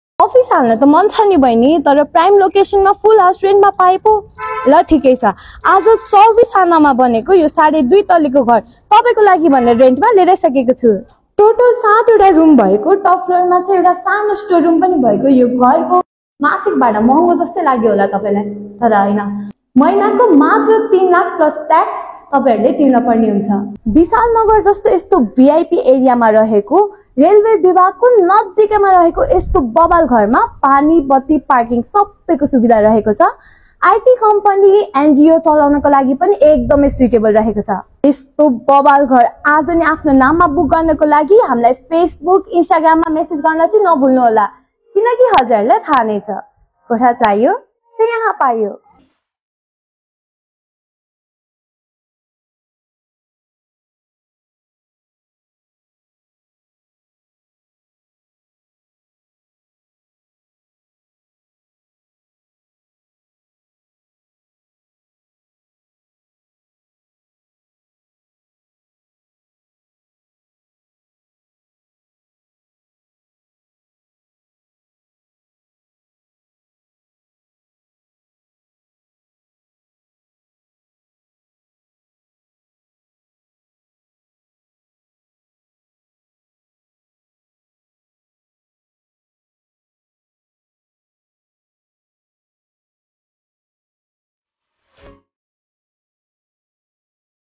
𝐇𝐮𝐠𝐞 𝐂𝐨𝐦𝐦𝐞𝐫𝐜𝐢𝐚𝐥 𝐒𝐩𝐚𝐜𝐞 𝐈𝐧 𝐏𝐫𝐢𝐦𝐞 𝐋𝐨𝐜𝐚𝐭𝐢𝐨𝐧.𝐃𝐞𝐭𝐚𝐢𝐥𝐬:-𝟐𝟒𝐀𝐧𝐧𝐚-𝟕𝐑𝐨𝐨𝐦𝐬-𝟏 𝐇𝐚𝐥𝐥 𝐋𝐨𝐜𝐚𝐭𝐢𝐨𝐧⬇️:𝐁𝐢𝐬𝐡𝐚𝐥𝐧𝐚𝐠𝐚𝐫 𝐌𝐨𝐧𝐭𝐡𝐥𝐲
For more information ⬇️
📞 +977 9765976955 | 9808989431 | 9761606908 | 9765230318
🏠 Samakhushi Chowk, Kathmandu

For Second hand bike or car buy and sell: GaadiChaiyo